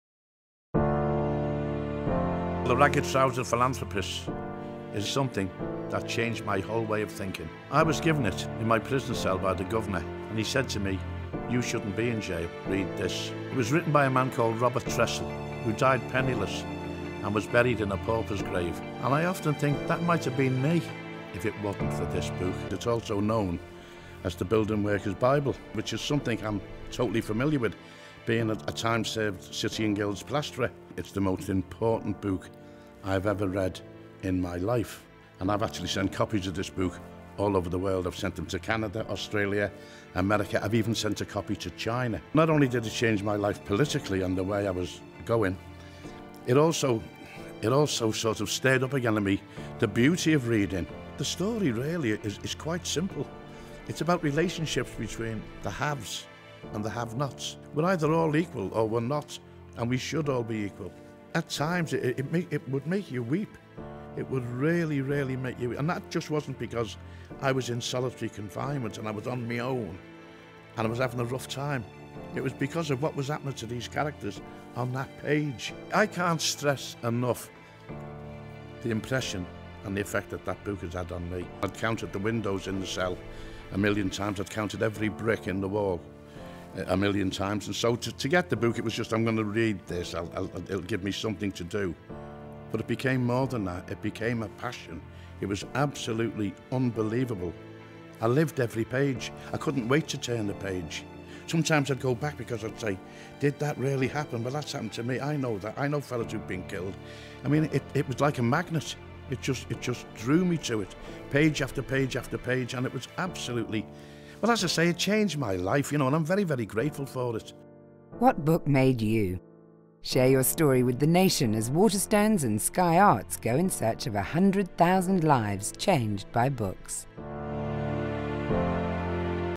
The Book That Made Me...Ricky Tomlinson on The Ragged-Trousered Philanthropists
"This book was given to me when I was in solitary confinement, by the prison governor. It's something that changed my whole way of thinking. It's the most important book I've ever read in my life. Not only did it change my life politically, it also stirred up again in me the beauty of reading."